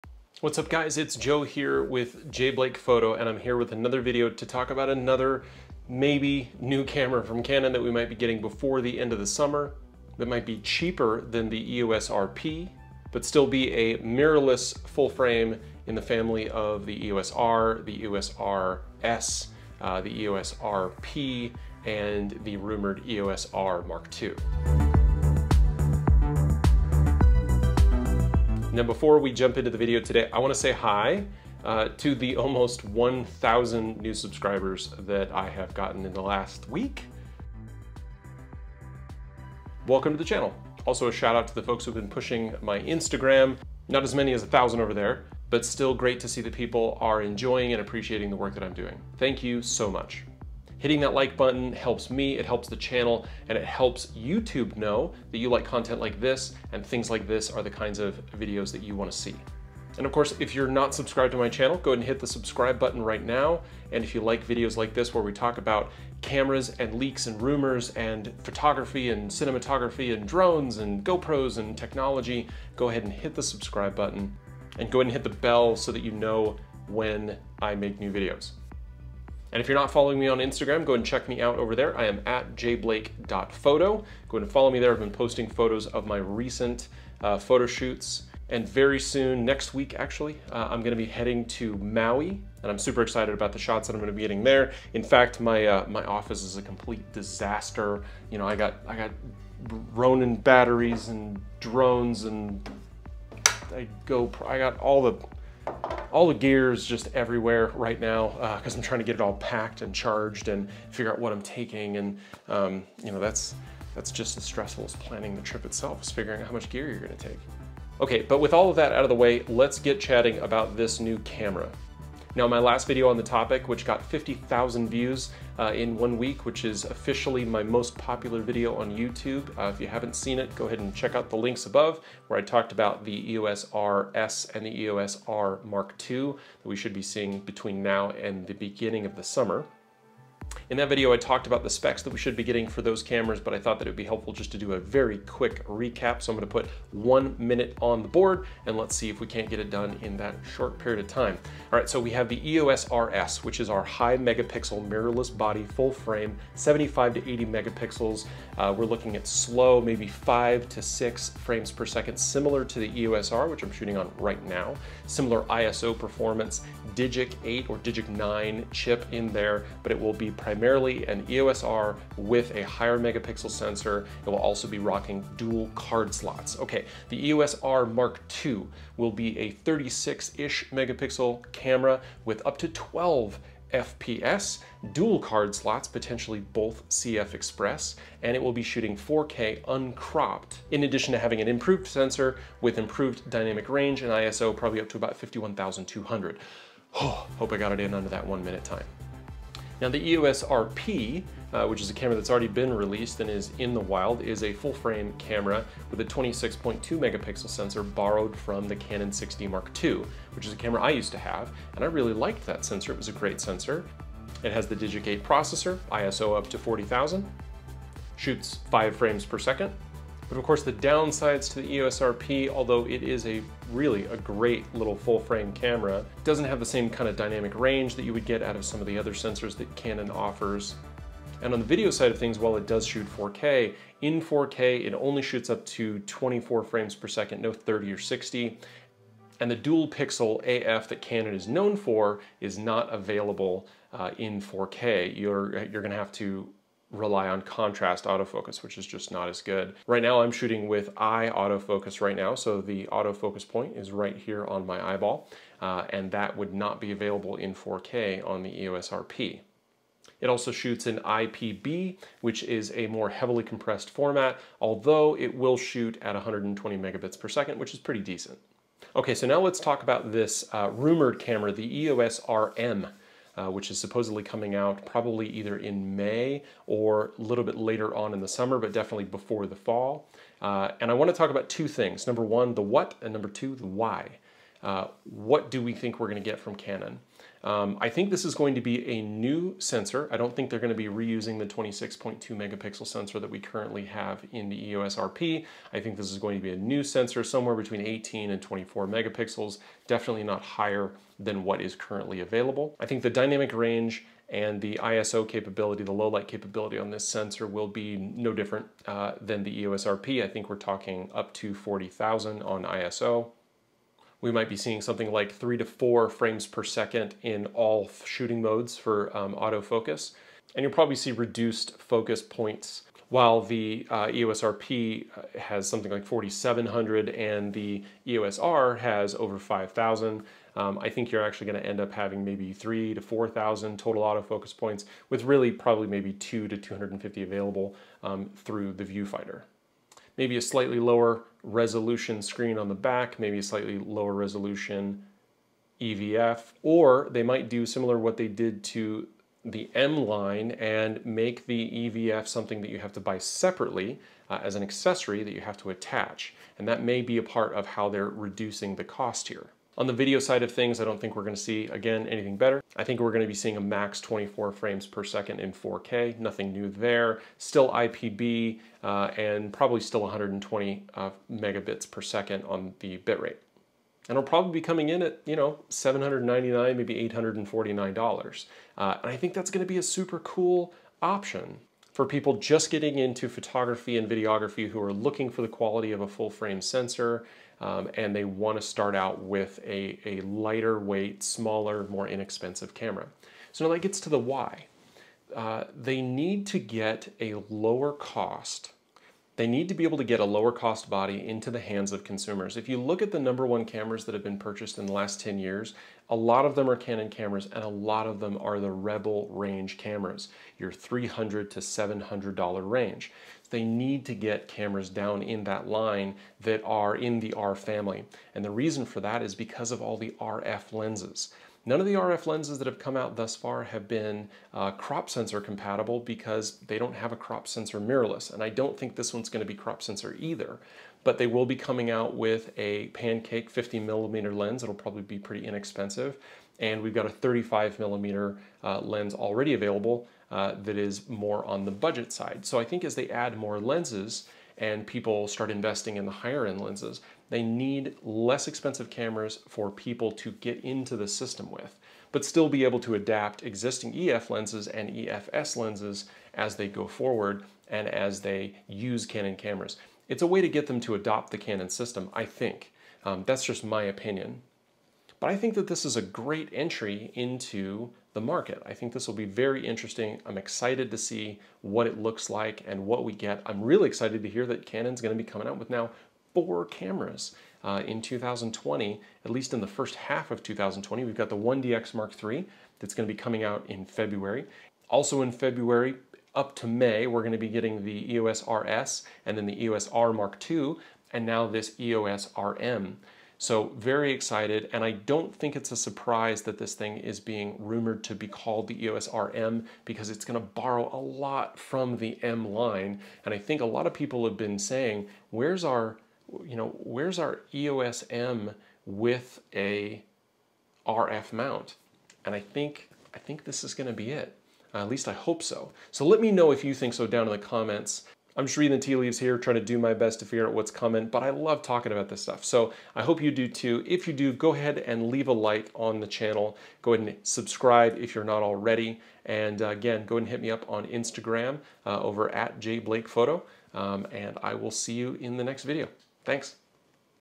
Canon EOS RM Rumors - 4 new Canon Cameras in 2020?
Looks like we are getting a budget RF mount EOS camera from Canon this year.